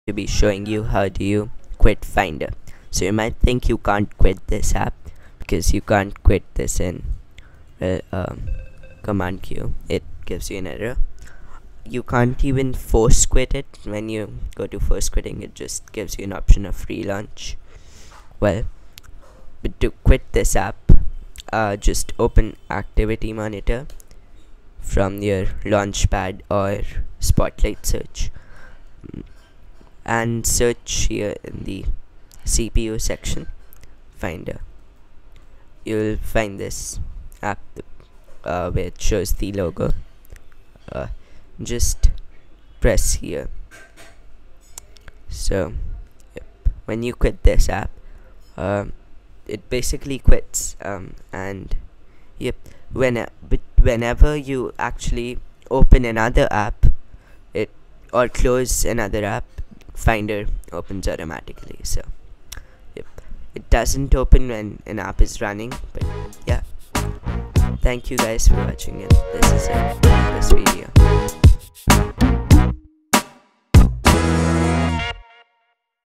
how to quit finder on Mac? (easy!)
In this video, I tell you how to quit finder on your Mac...

it seems like you can't but well you can!